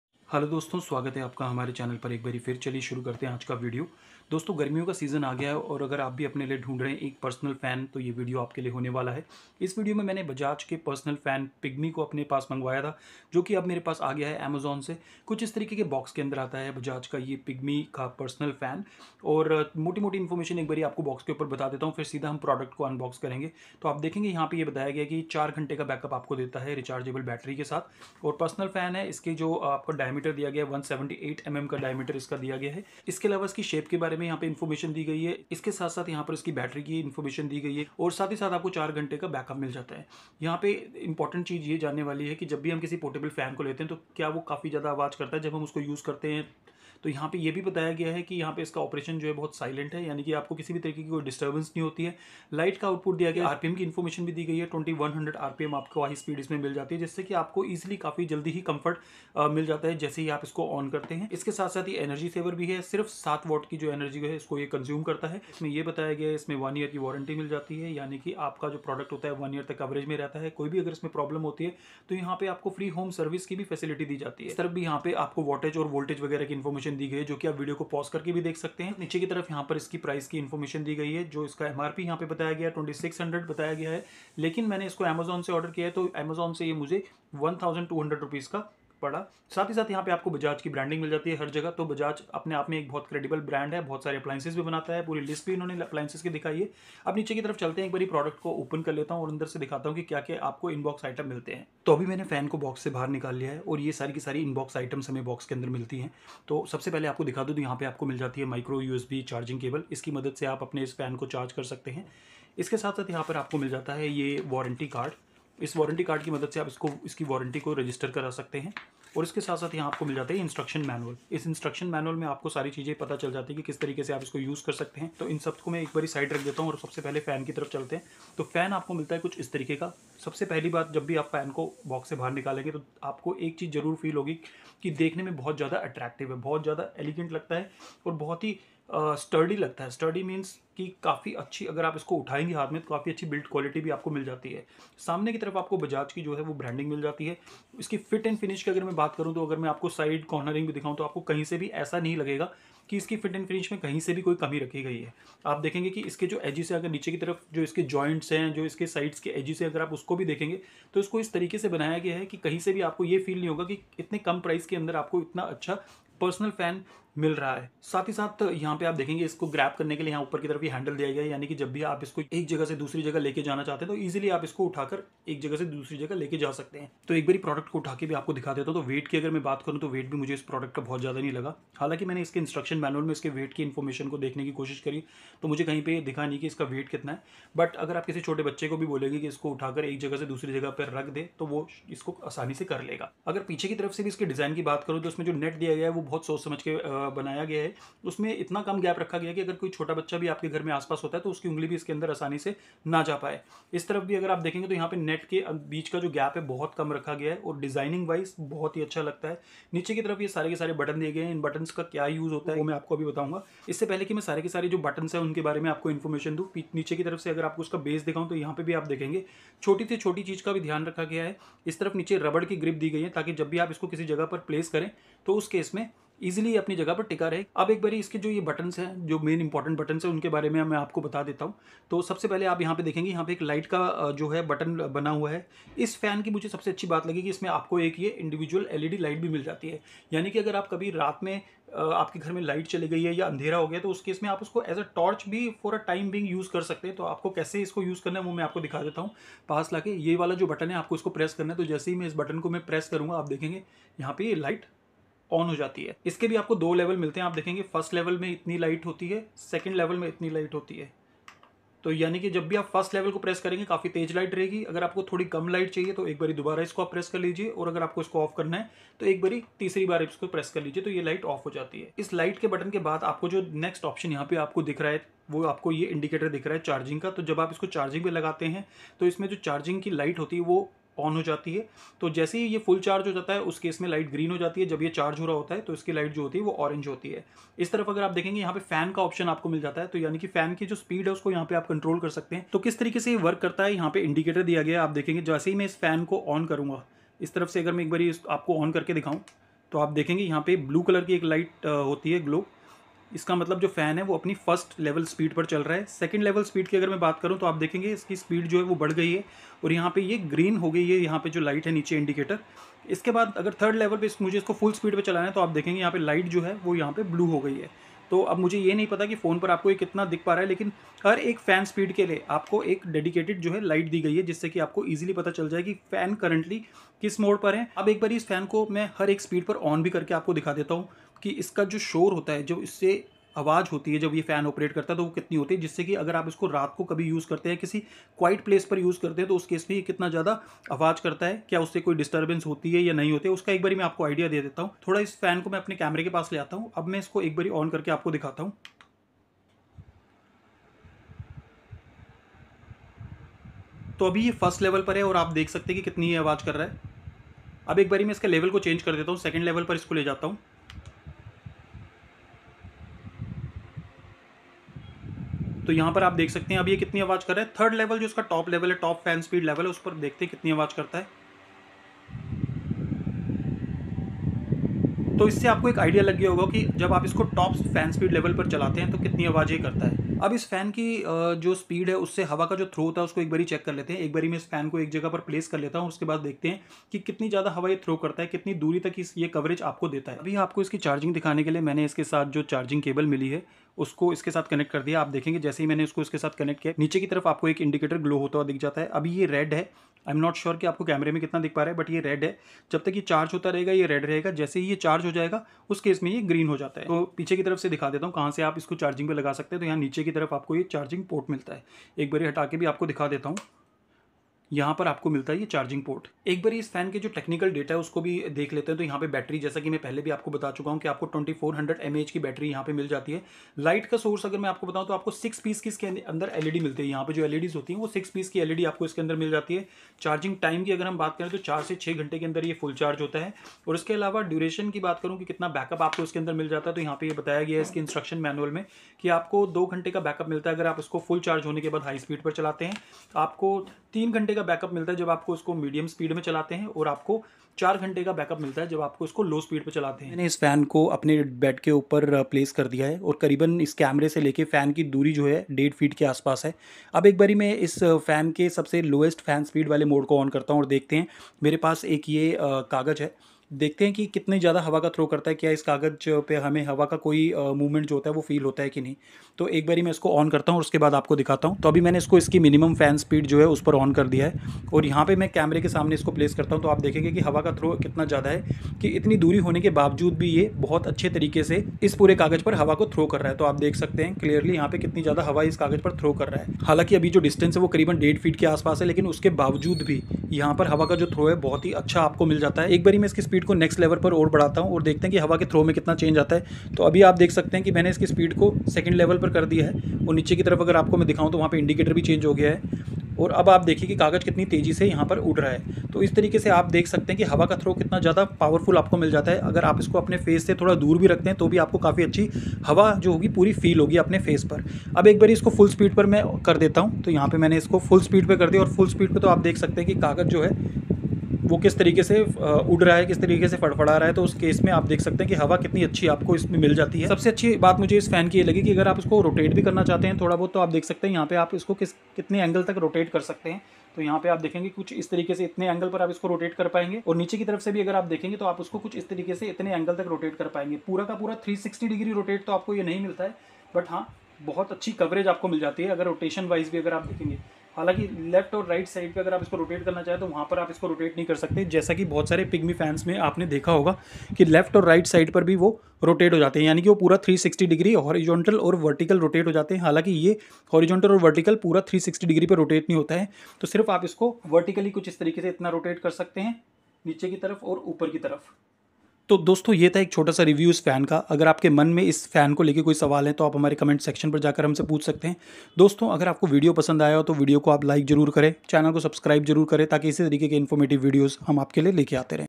Bajaj Pygmy Go 178mm Personal Fan Unboxing, Demo & Review | Best Portable Rechargeable Personal Fan
Hi Friends In this video we are showing you the unboxing and review of Bajaj pigmy go 178 mm portable rechargeable fan. Hope you enjoy this video!!!

Bajaj Pygmy Go 178 mm Blue Personal Fan

Bajaj PYGMY 178mm White Personal Fan

Bajaj PYGMY MINI 110mm Personal Fan